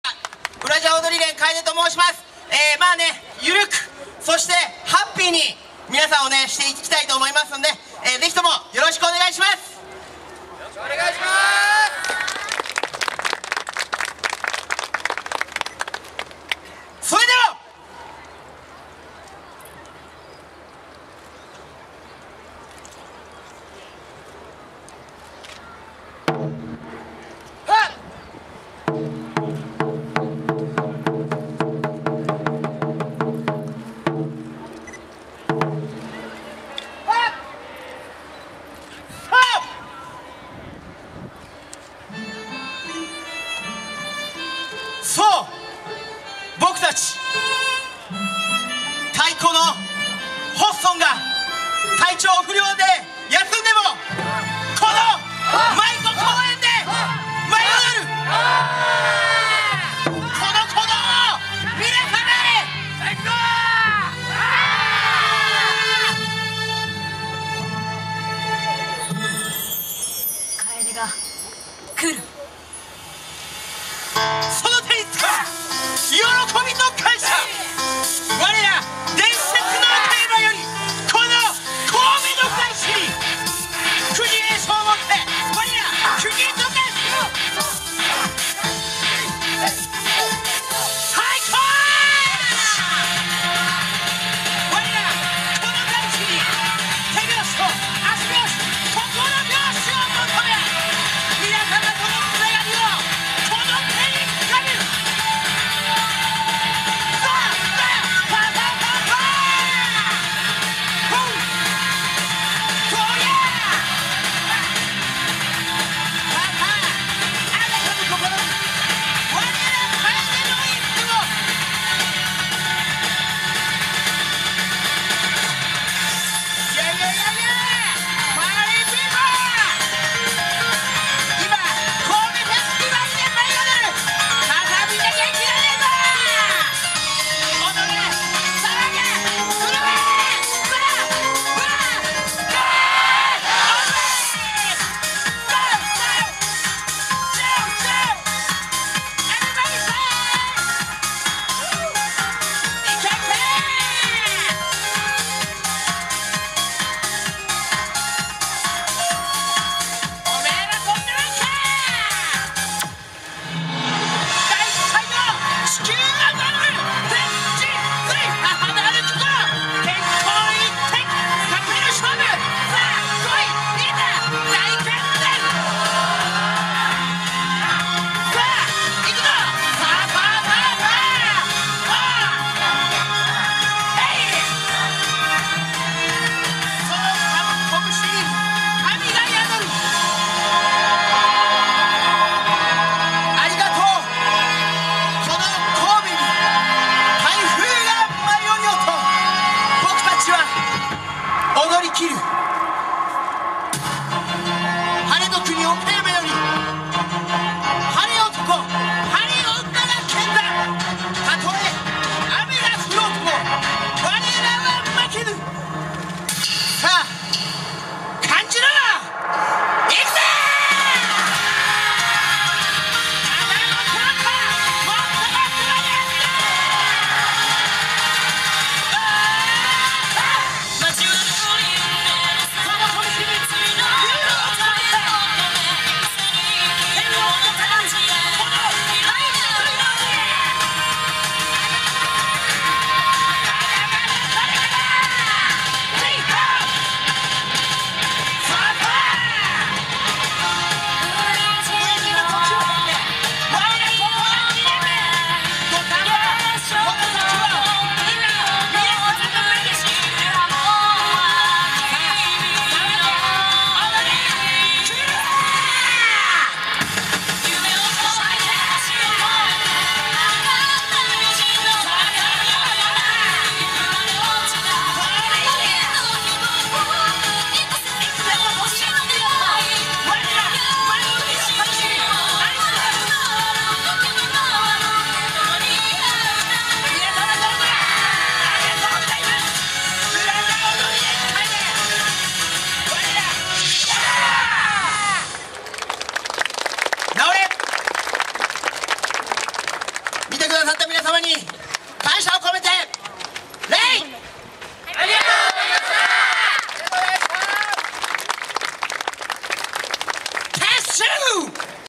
「うらじゃ踊り連 楓」 神戸よさこいまつり2016
兵庫県神戸市で9月2～4日で開催の本祭2日目舞子公園会場での（14時頃まで）記録です。